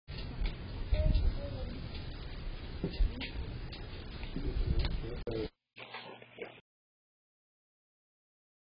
Blocking Driveway
A family of three was seen blocking the driveway and spending time.